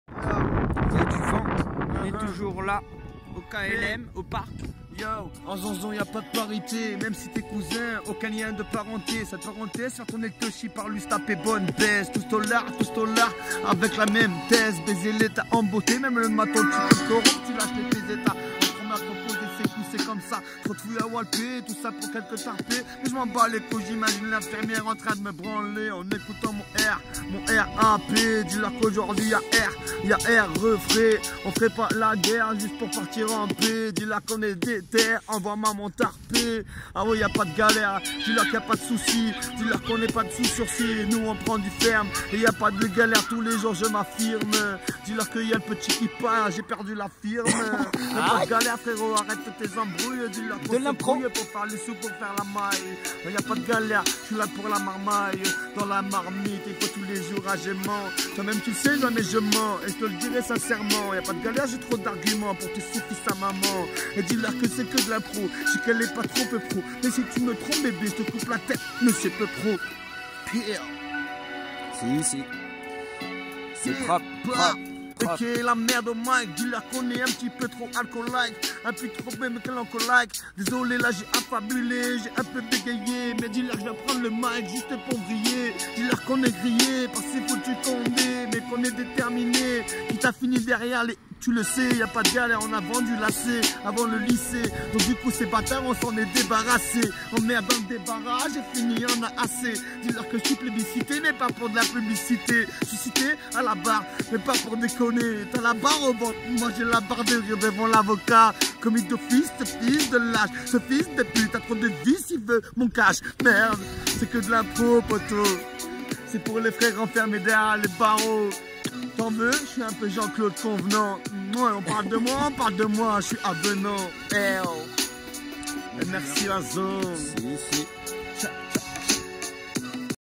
Freestyle avec le poto HSB trakma rappeur du herps!
Après la soirée du seigneur le dimanche à vous faire partager du son... et merci